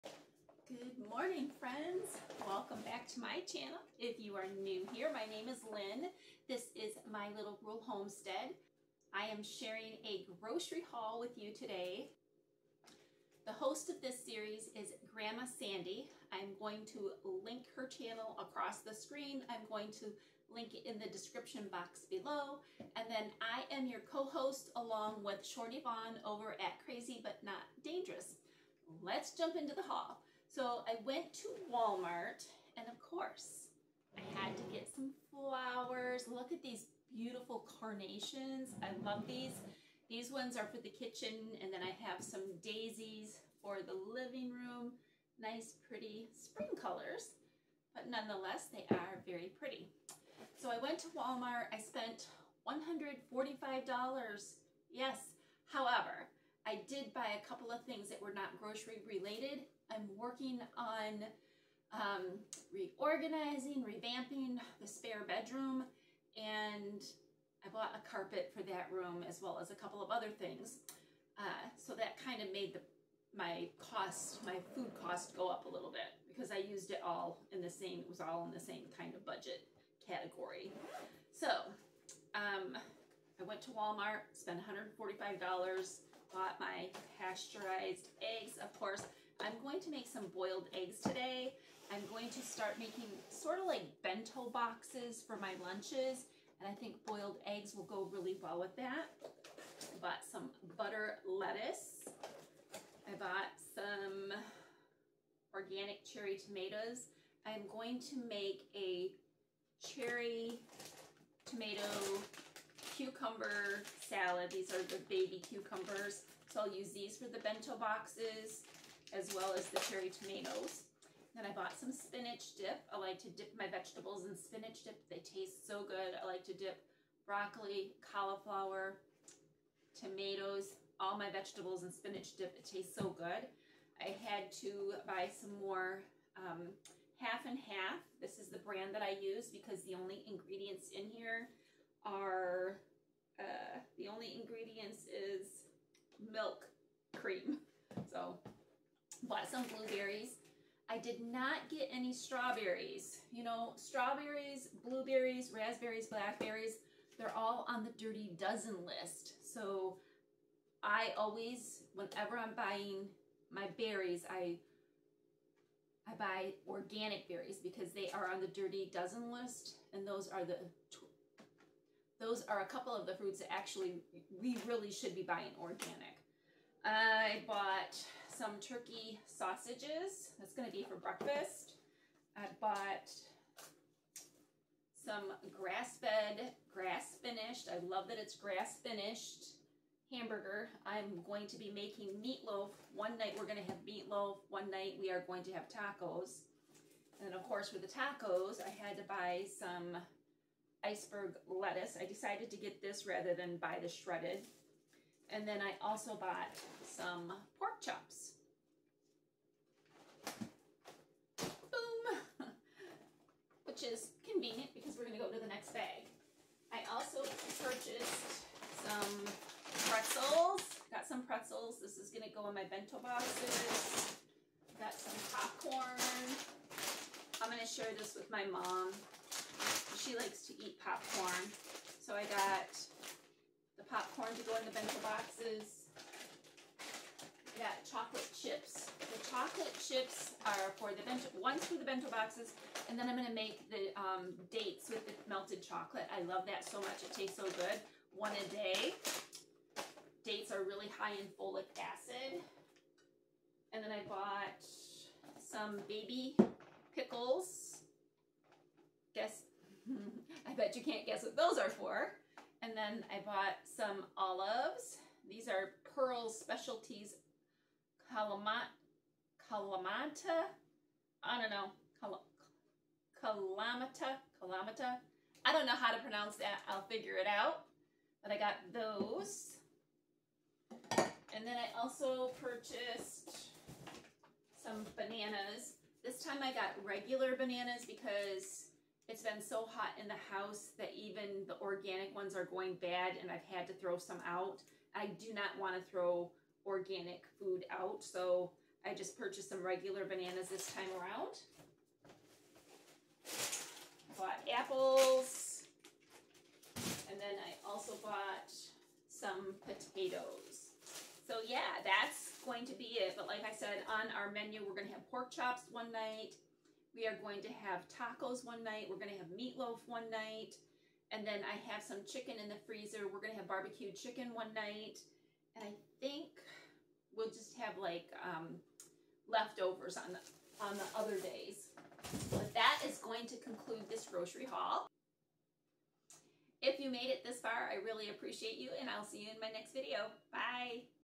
Walmart Grocery Haul/Pantry Restock/Grandma Sandy
Total $145.07, yes I bought some command strips and a throw rug/carpet for
the spare room.

Our menu plan is:
Sunday:  Shredded BBQ Chicken
Monday:  Meatloaf
Tuesday:  Chicken
Wednesday:  Pork Chops
Thursday:  Meatballs
Friday:  Tacos
Saturday:  Leftovers